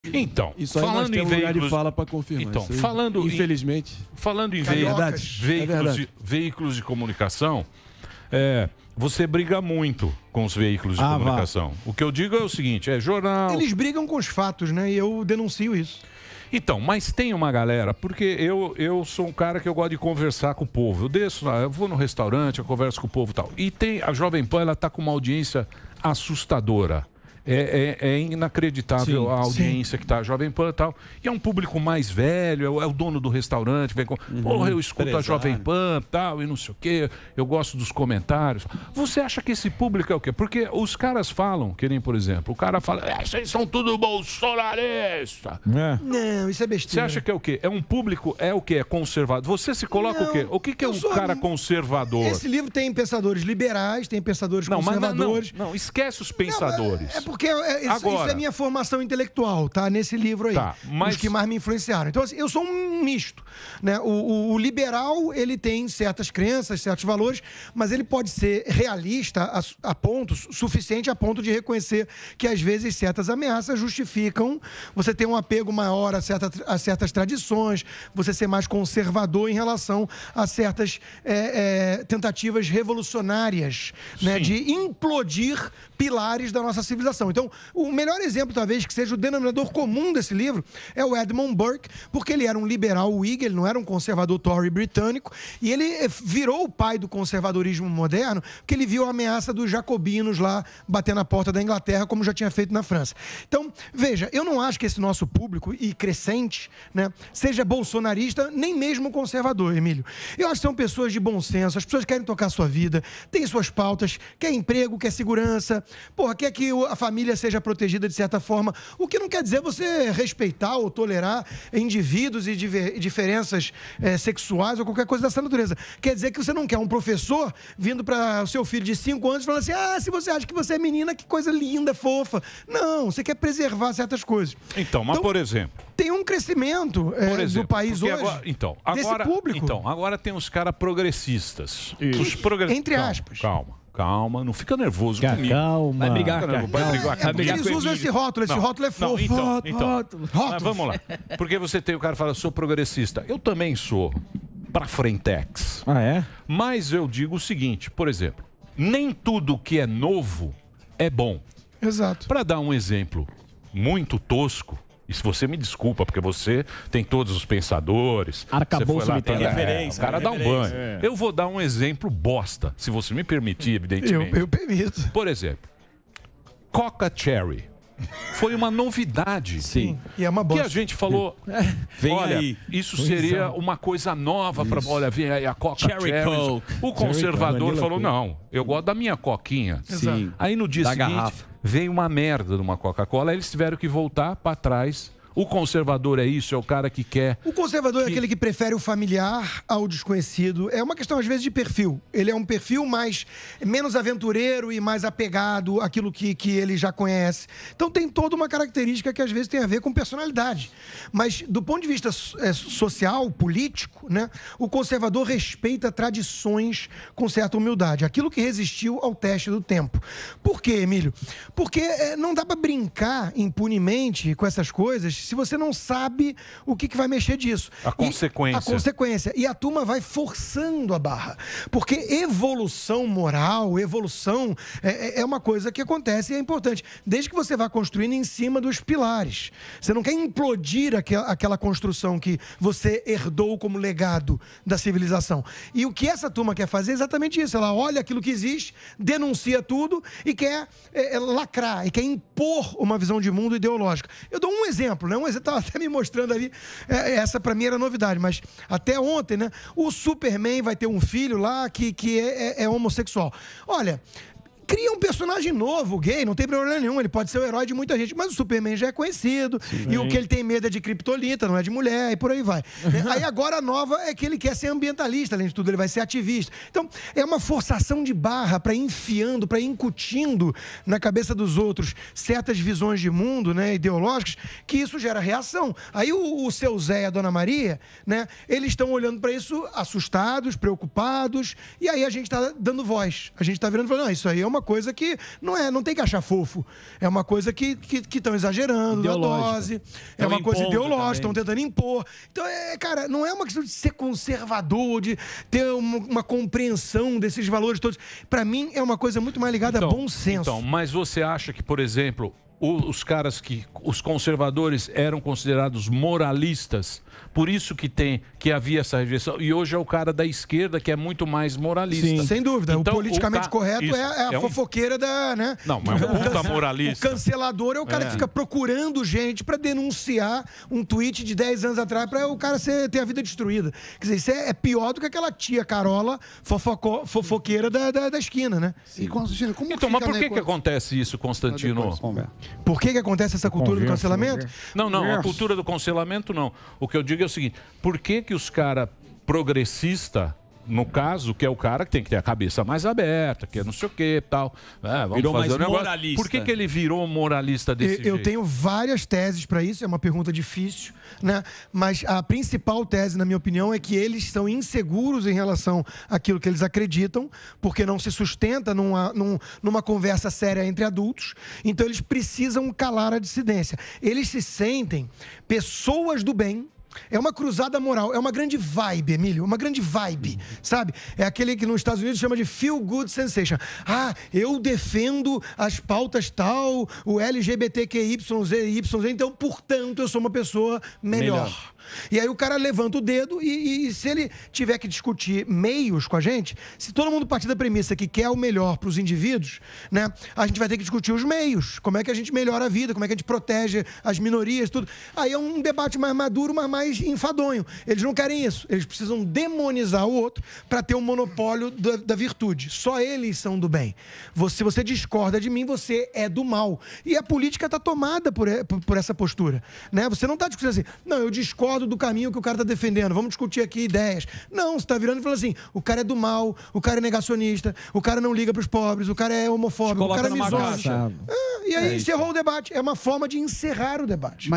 Rodrigo Constantino: 'PROGRESSISTAS QUEREM LACRAR IMPONDO SUA PRÓPRIA VISÃO IDEOLÓGICA'
Rodrigo Constantino explica as diferenças entre os conceitos de liberal e conservador, mencionados em seu livro "Pensadores da Liberdade", e critica a imposição da visão de mundo dos considerados progressistas sem respeitar as tradições da sociedade